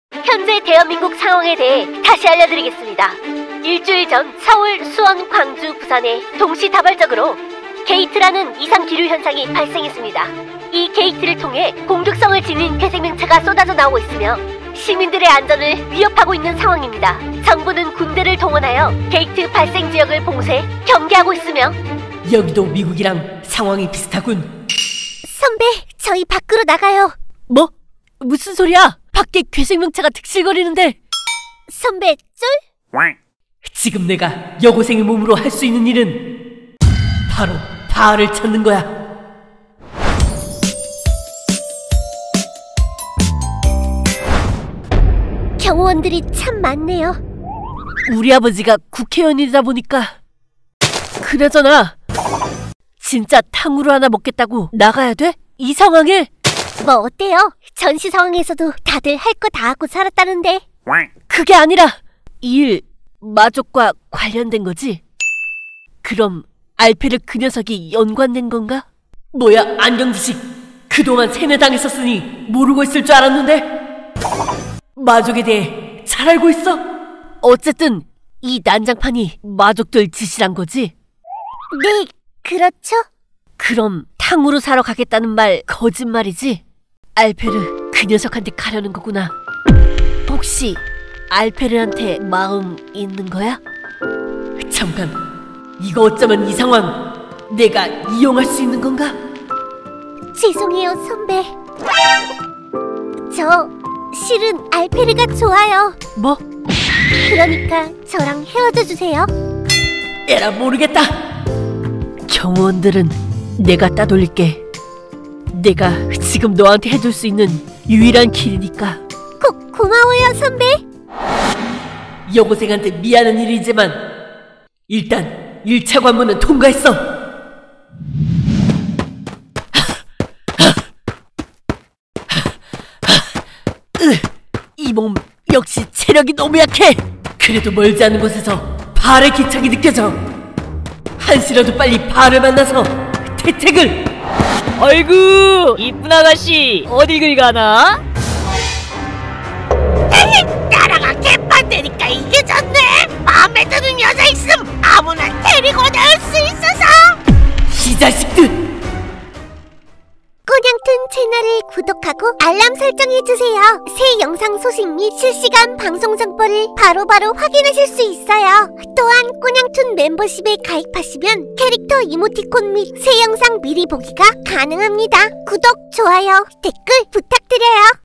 경호원 4명이 철벽 감시하는 집에서 탈출하는 법 [서열학교 Z]
- 시나리오: 김초멜｜- 작화: 물뭉이｜- 편집: 김초멜